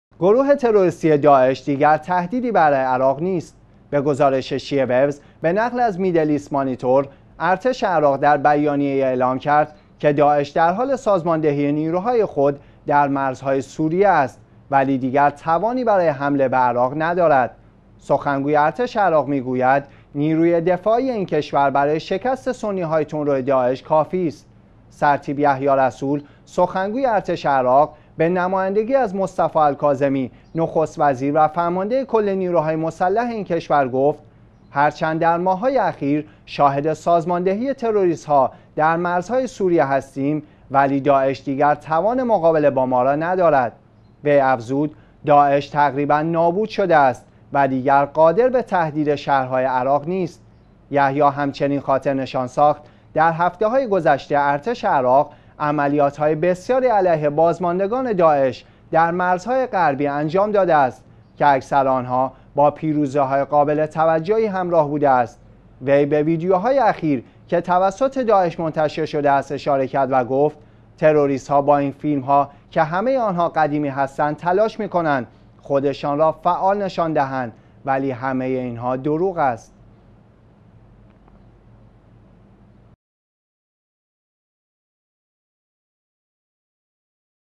نماینده الکاظمی در ارتش: داعش دیگر تهدیدی برای عراق نیست
گروه تروریستی داعش دیگر تهدیدی برای عراق نیست.
به گزارش شیعه ویوز به نقل از میدل ایست مانیتور، ارتش عراق در بیانیه ای اعلام کرد که داعش در حال سازماندهی نیروهای خود در مرزهای سوریه است ولی دیگر توانی برای حمله به عراق ندارد.
سخنگوی ارتش عراق می گوید نیروی دفاعی این کشور برای شکست سنی های تندروی داعش کافی است.
سرتیپ یحیی رسول، سخنگوی ارتش عراق به نمایندگی از مصطفی الکاظمی نخست وزیر و فرمانده کل نیروهای مسلح این کشور گفت: هرچند در ماه های اخیر شاهد سازماندهی تروریست ها در مرزهای سوریه هستیم ولی داعش دیگر توان مقابله با ما را ندارد.
وی افزود: داعش تقریباً نابود شده است و دیگر قادر به تهدید شهرهای عراق نیست.
یحیی همچنین خاطرنشان ساخت: در هفته های گذشته ارتش عراق عملیات های بسیاری علیه بازماندگان داعش در مرزهای غربی انجام داده است که اکثر آنها با پیروزی های قابل توجهی همراه بوده است.
وی به ویدئوهای اخیری که توسط داعش منتشر شده است، اشاره کرد و گفت تروریست ها با این فیلمها که همه آنها قدیمی هستند، تلاش می کنند خودشان را فعال نشان دهند ولی همه اینها دروغ است.